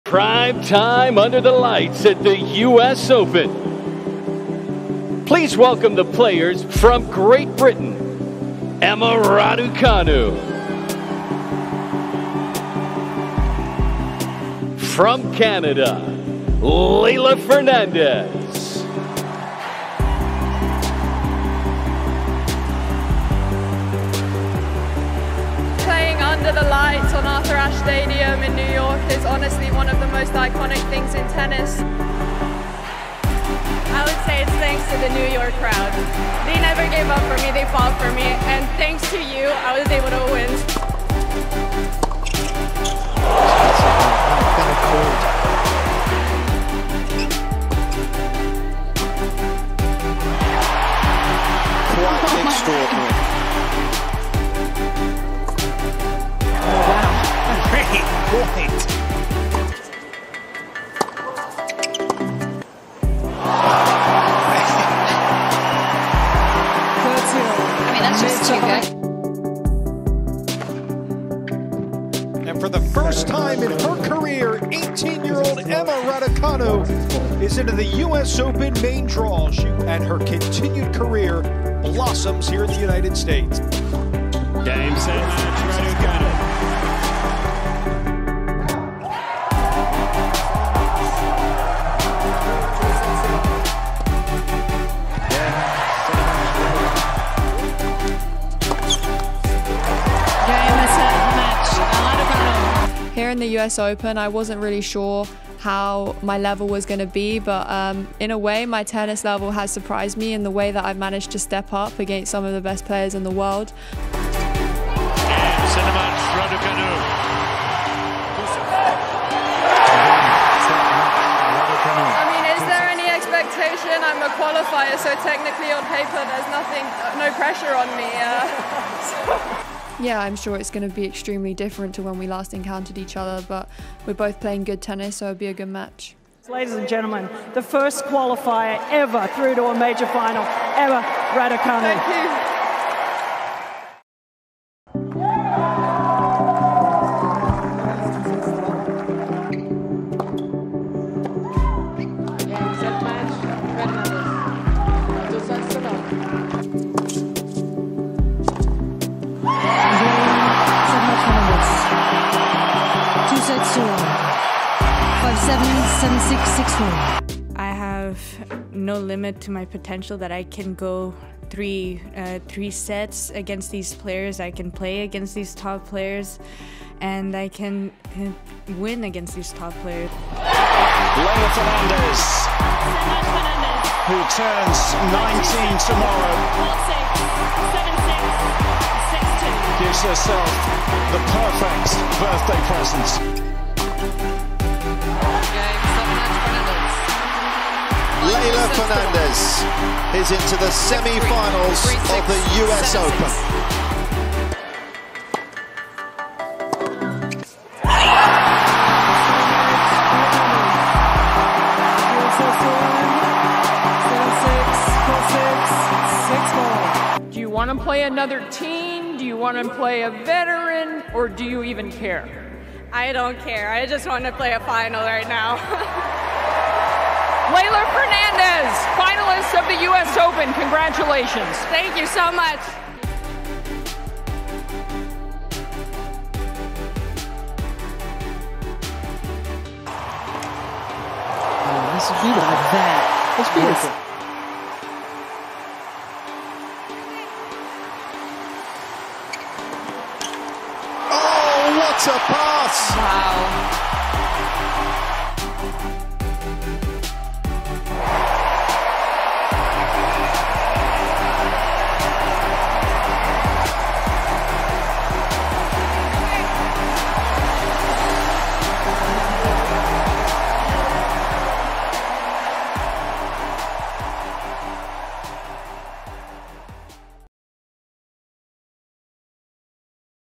Emma Raducanu vs Leylah Fernandez - Road to the Final | 2021 US Open
18-year-old Emma Raducanu and 19-year-old Leylah Fernandez have battled their way to the final of the 2021 US Open. So let's have a look at how they made it to this year's showpiece event.